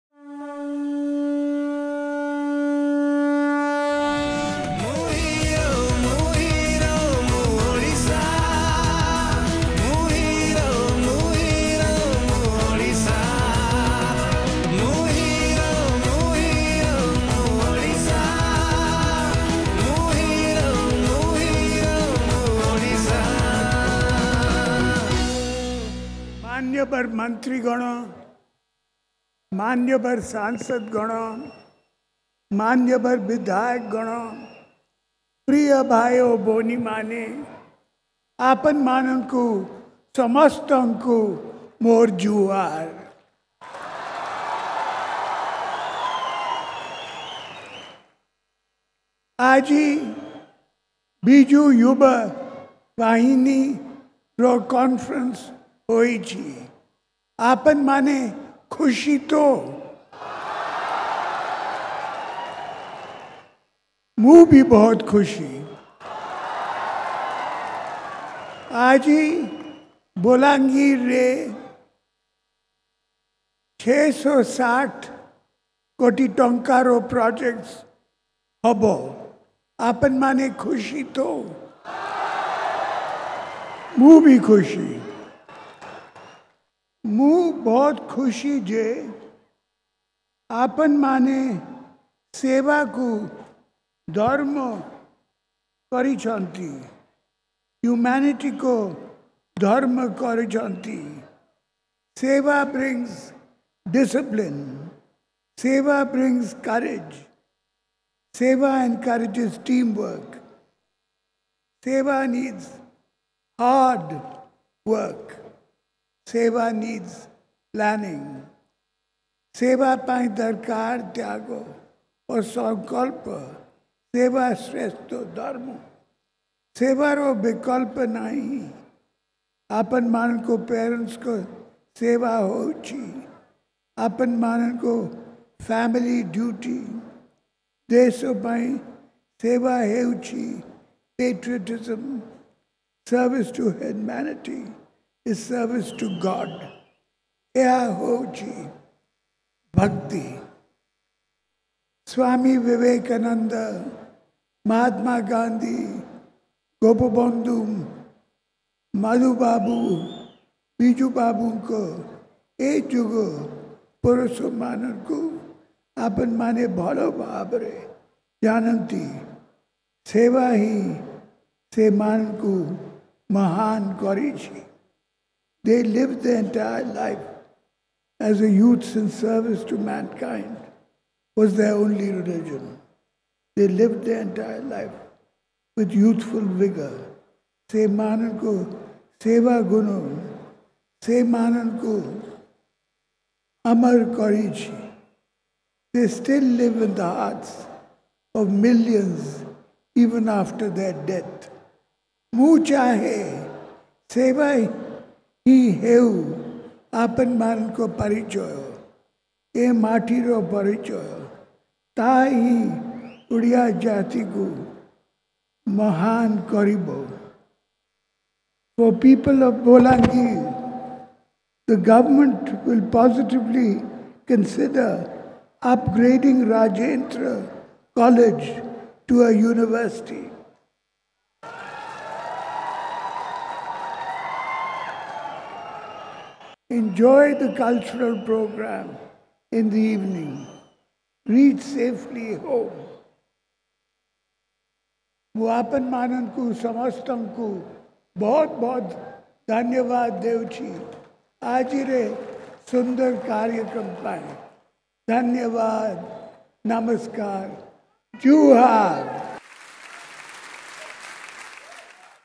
Youth Conclave Celebrating Volunteerism | CM Speech @ Balangir
CM Naveen Patnaik at Biju yuva Vahini's Youth Conference which was held in Balangir, Odisha

For always being informed, and entertained find us here:

An OdishaLIVE Web Production.

Bhubaneswar-751017 (Odisha).